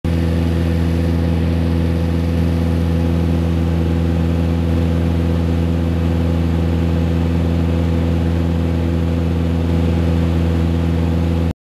Shutting the Prop Off Through Camera Settings in Mooney Airplane
I'm tired of the annoying prop flicker in some of my older videos so I've been experimenting with camera settings to remove the prop effect and am quite pleased with the results. Check out this video as I lower the shutter speed and the prop virtually disappears.